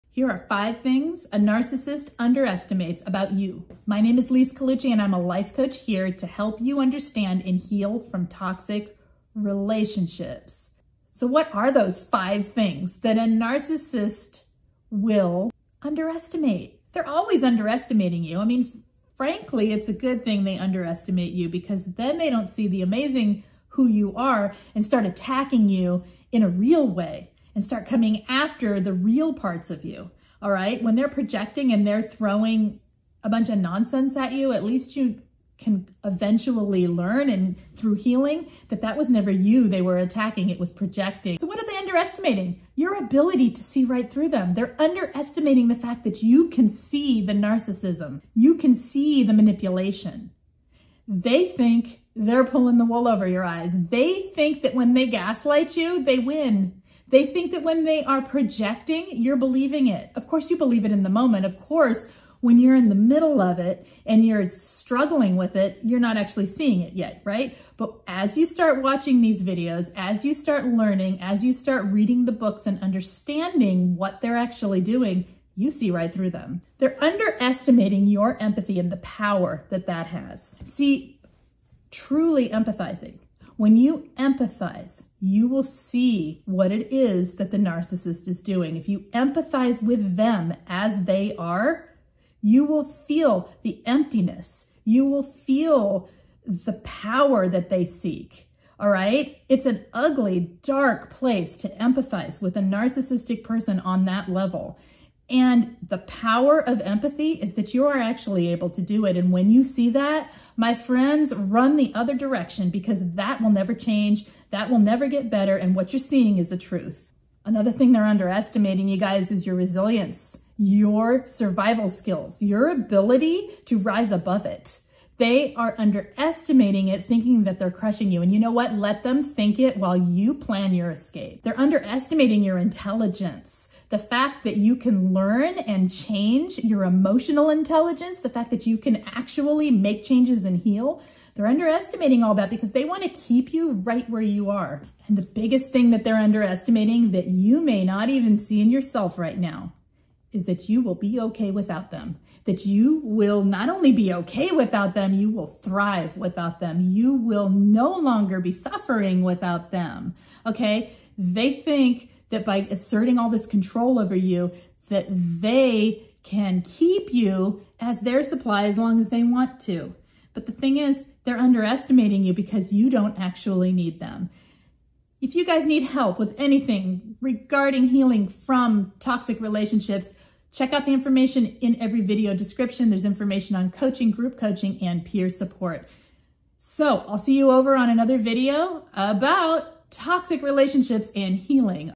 The Narcissist Underestimates You
Ever felt underestimated by a narcissist? This video tells the five key ways narcissists think you're less than you are and why they're completely wrong. Join Lise Colucci, a dedicated life coach in narcissistic abuse recovery, as she explores how these underestimations can highlight your strengths and offer paths to empowerment.

Discover how to turn the narcissist's misjudgments into your advantage, reinforcing your self-worth and resilience.

This video is about the Top 5 Traits Narcissists Can't Recognize In You: Reclaim Power. But It also covers the following topics:

Overcoming Narcissistic Views
Top 5 Narcissist Underestimation
Empowering Victims

Video Title: Top 5 Traits Narcissists underestimates About You | LiseColucci.com